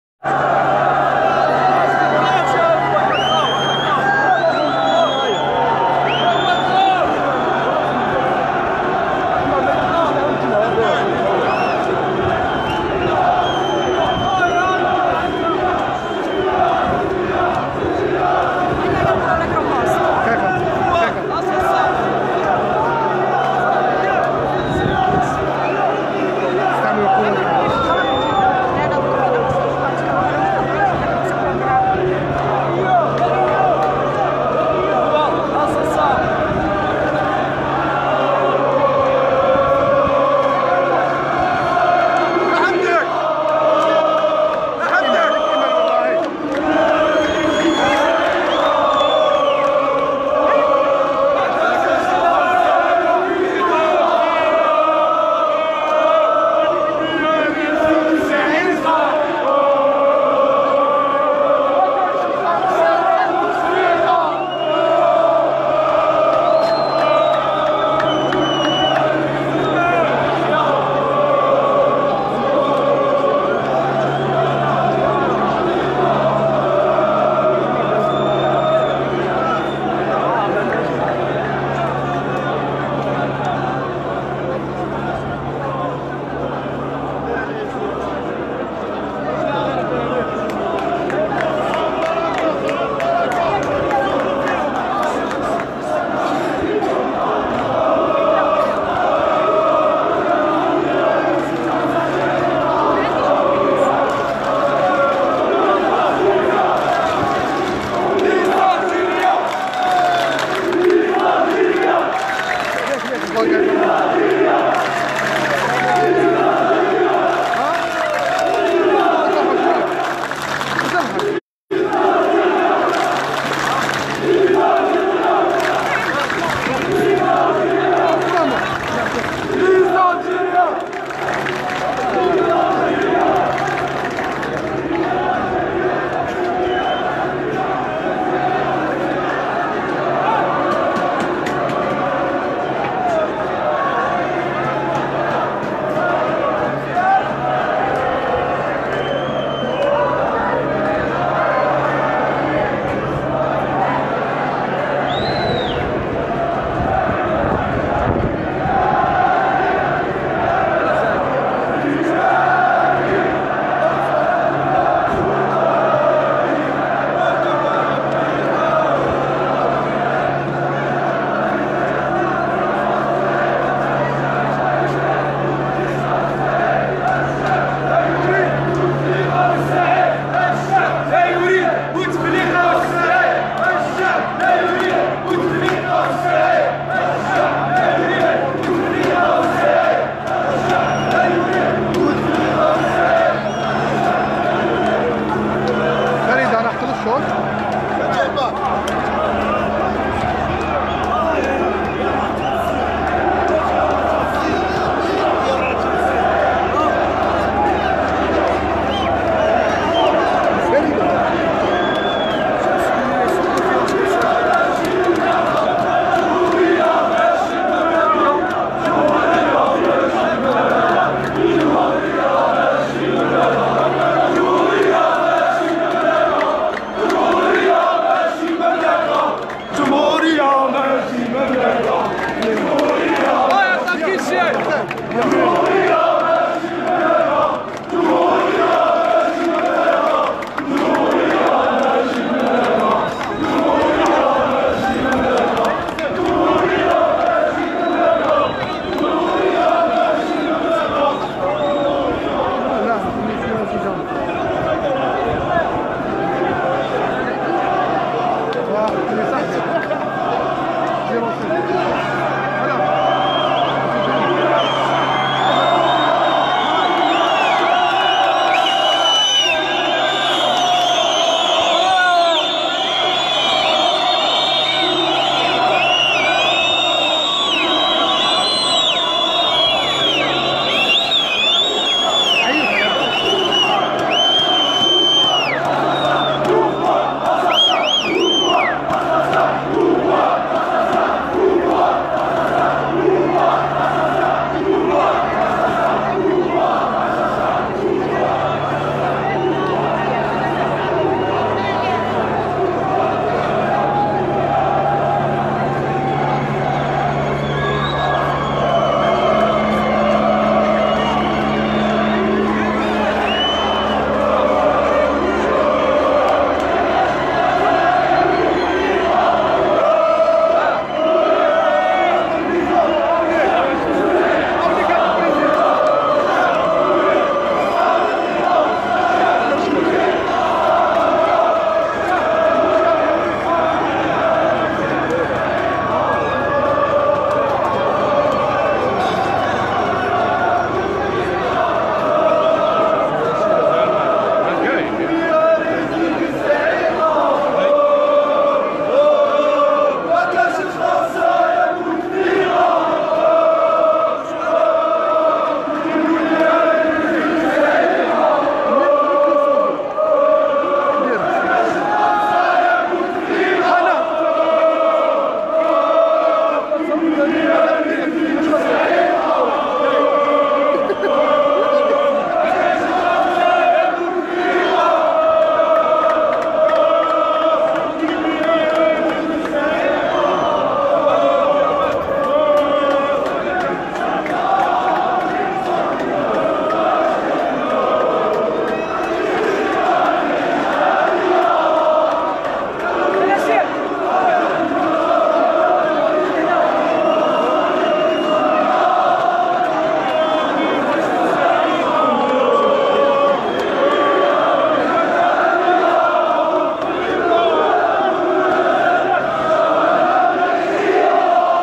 مليونية الجمعة 1 مارس 2019 المظاهرات السلمية بث مباشر ضد العهدة 5 الجزائر العاصمة
✌إذا أعجبك الڤيديو لا تبخل علي بلايك للڤيديو ✌
و الإشتراك في القناة وتفعيل زر الجرس للمزيد من الإبداع
فيديو عبارة عن بث مباشر لمظارات سلمية 1 مارس 2019 الجزائر العاصمة
سلمية سلمية في انتظار باقي الولايات ربي يحفظ البلاد